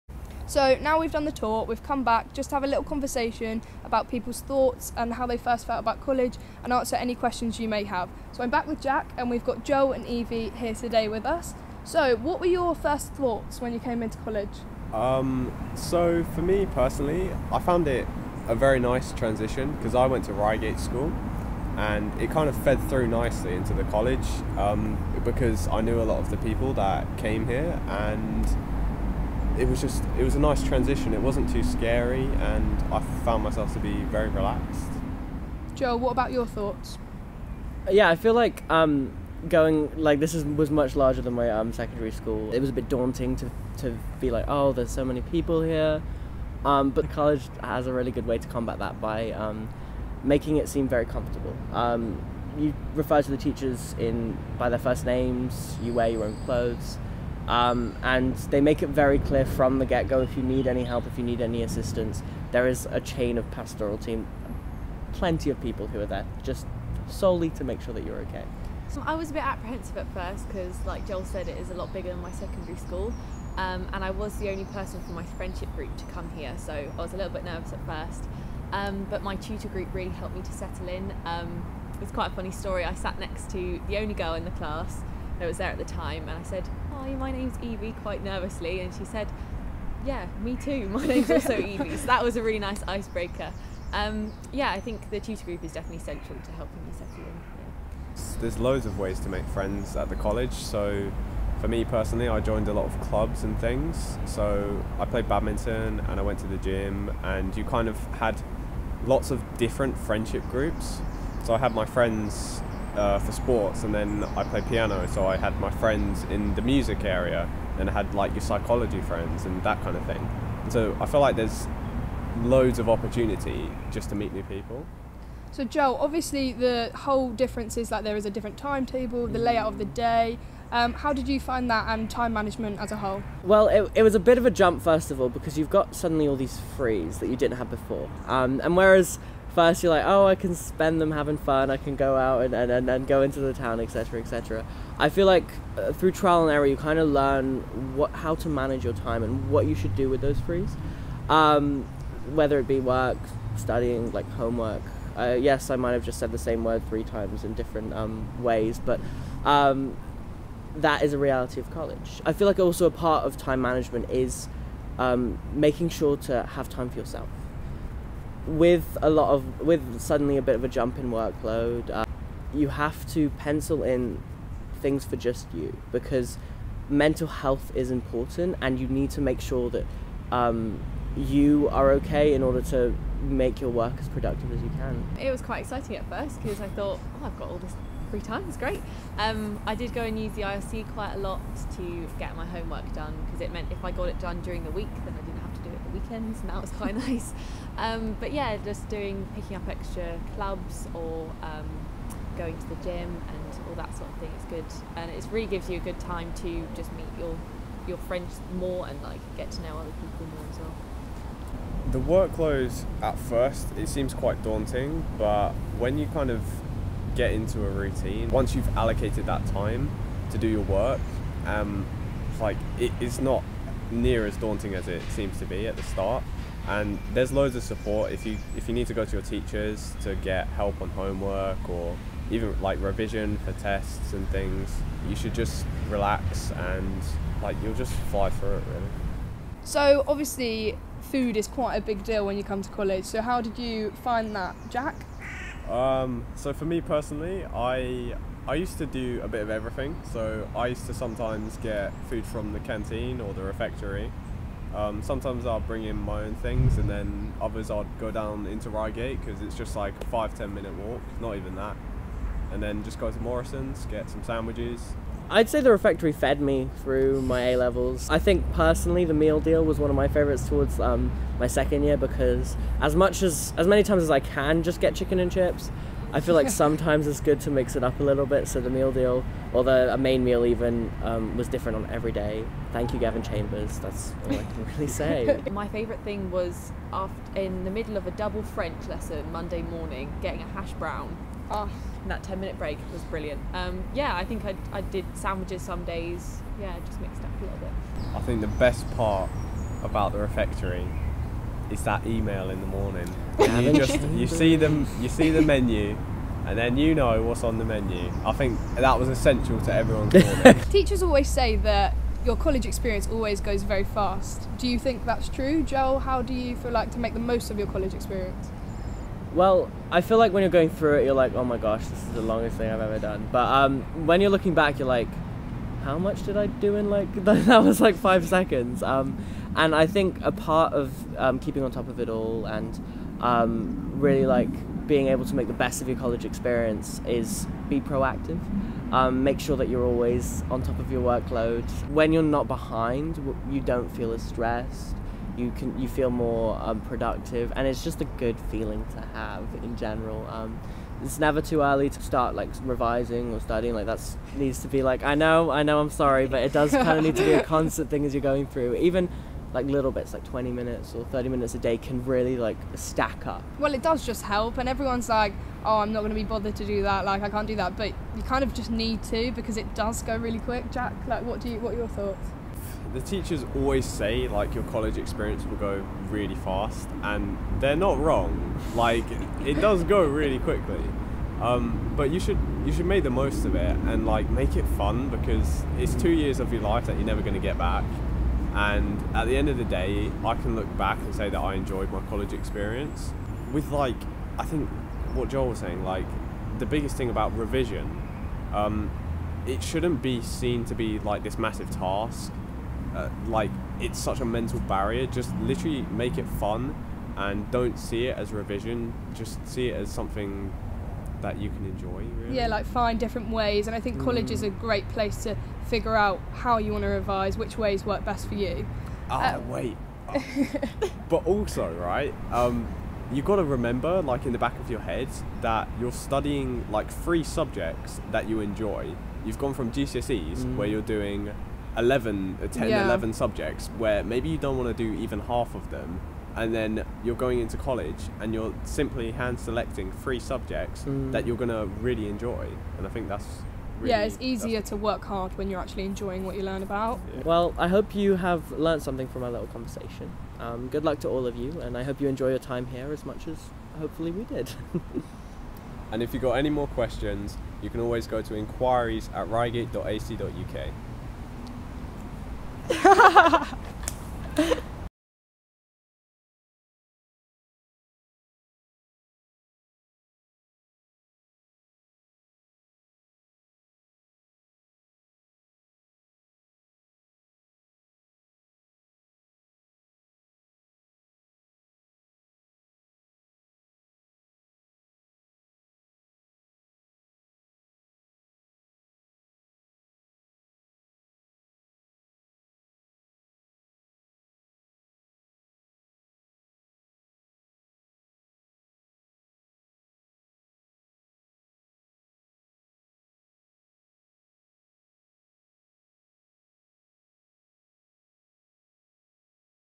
College Life
Four Reigate College students talk about how easy it is to settle in and what it's like to be a student at Reigate College.